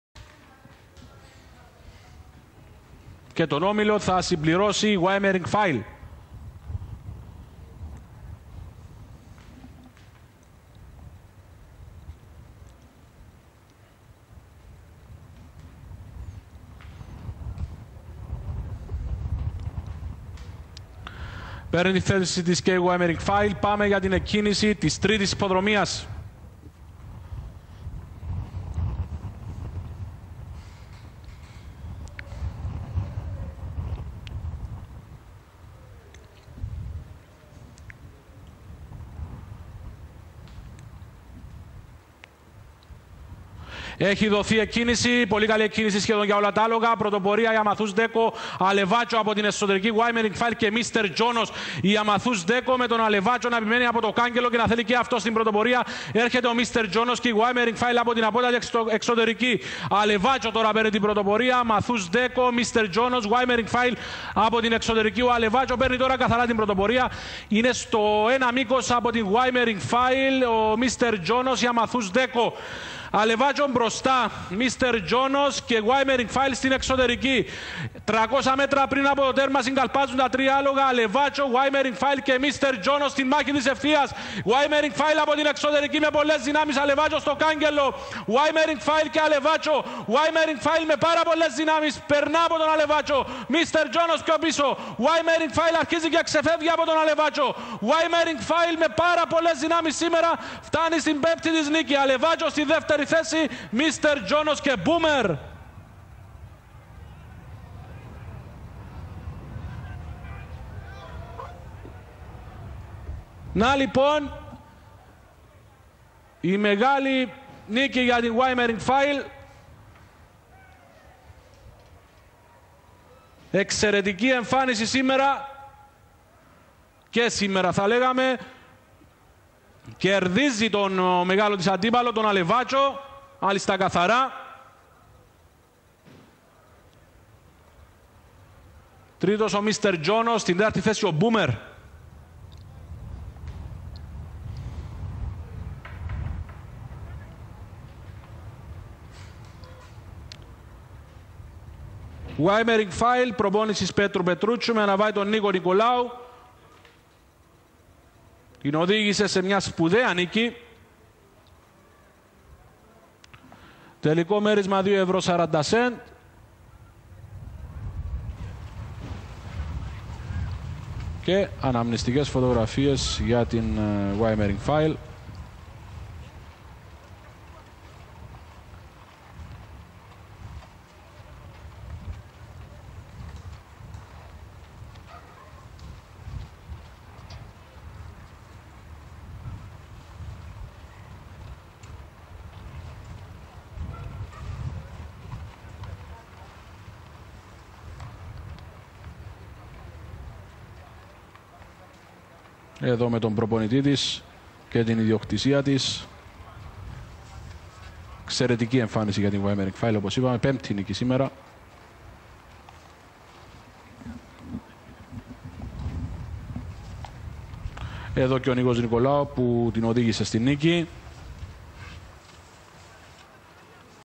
WYMERING FILE - 5 FURLONG (24 SEP 2008)
Απόσταση: 1000 μέτρα
 Χρόνος     :   0.59.43

Ιδιοκτήτης:  κ. Ράντυ Γουέητ Κάρτερ
Αναβάτης :  Ν.Κ. Νικολάου
Προπονητής :  Π. Πετρούτσιος